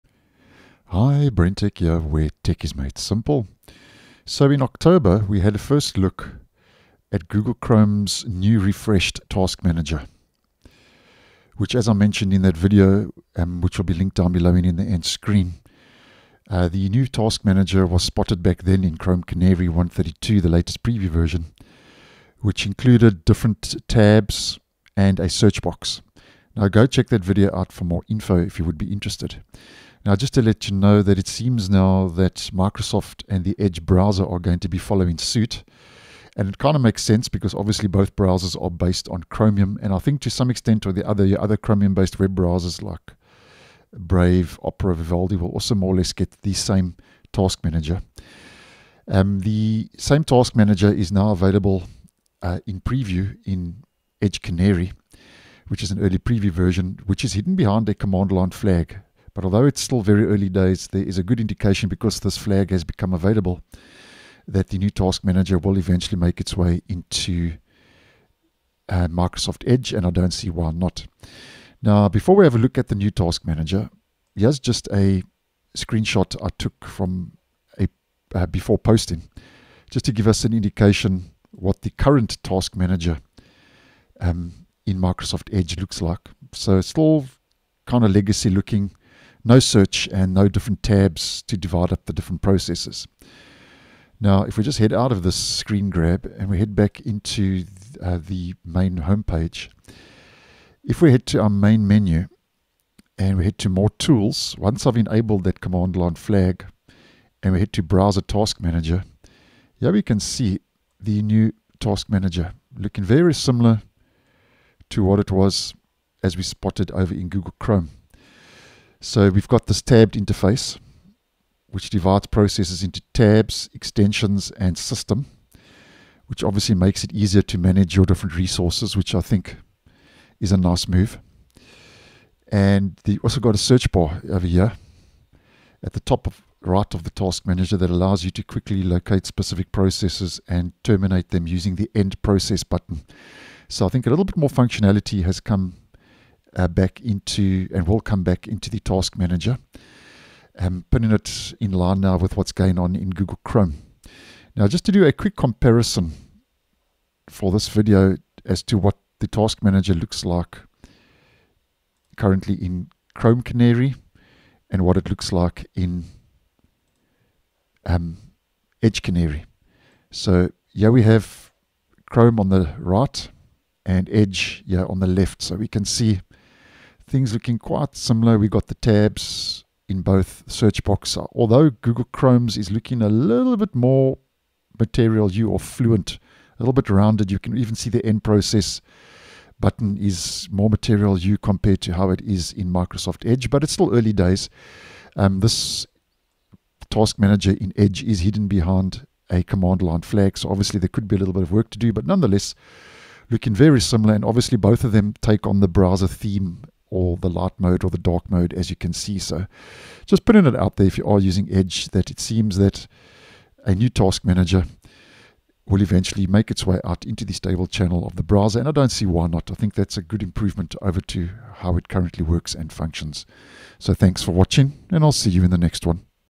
First Look at Microsoft Edge's New Chrome-Like Task Manager
In Edge Canary 133, the task manager now has tabs and a search box.
Enable using this command line flag: --enable-features=TaskManagerDesktopRefresh